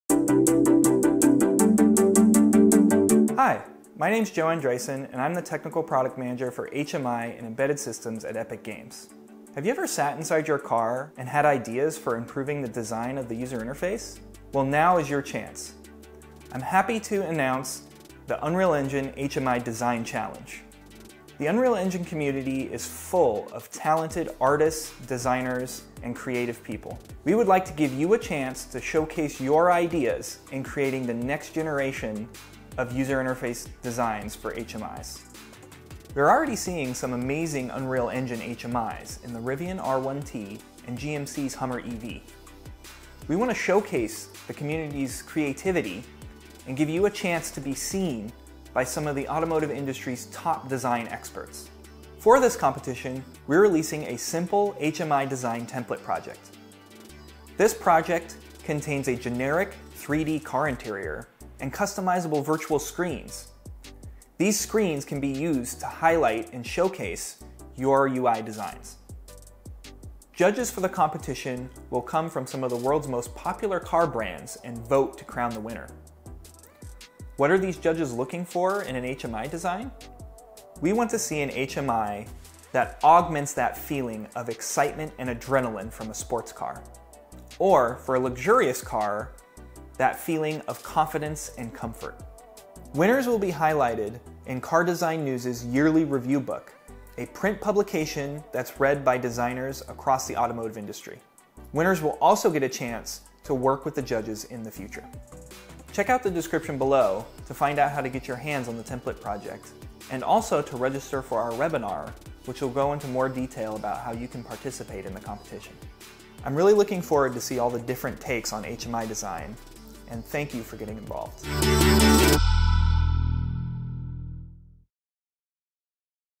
언리얼 엔진 HMI 디자인 챌린지에 참가하세요.
에픽게임즈에서 주최하는 첫 번째 HMI(Human-Machine Interface) 디자인 챌린지에 참가해 보세요!
게임, 영화, 건축 등 HMI 디자인 경험은 물론 자동차 산업에 대한 경험이 전혀 없어도 누구든지 참여할 수 있는 이번 챌린지에서는 실시간 기술을 활용하여 여러분만의 창의적인 디자인 콘셉트를 선보일 수 있으며, GM, 메르세데스 벤츠, 리비안, 현대자동차 등 세계에서 가장 혁신적인 자동차 회사들이 심사위원으로 참여하여 우승자에게 다양한 혜택을 제공합니다. 보다 자세한 내용은 영상 및 블로그에서 확인해 보세요.

- 챌린지 기간: 2022년 1월 26일 – 2022년 3월 31일 (미국 동부 시간)
- 웨비나 다시보기: 언리얼 엔진의 새로운 HMI 디자인 템플릿 알아보기 (한국어 공개 예정)